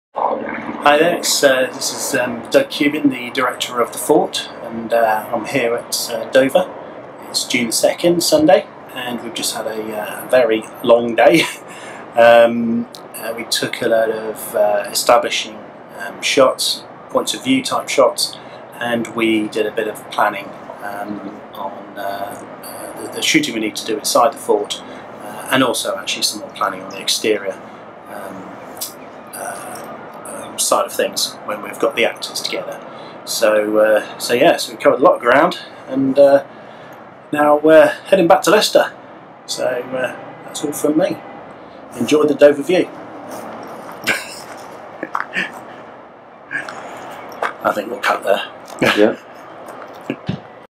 The Fort Video Blog - Sunday 2nd June 2013
A trip down to Dover to shoot some exterior shots in the sunshine. Also did a great deal of further planning. Lots and lots of planning. Then more planning. Then talking about the planning all the way home. Features don't make themselves you know!!  :)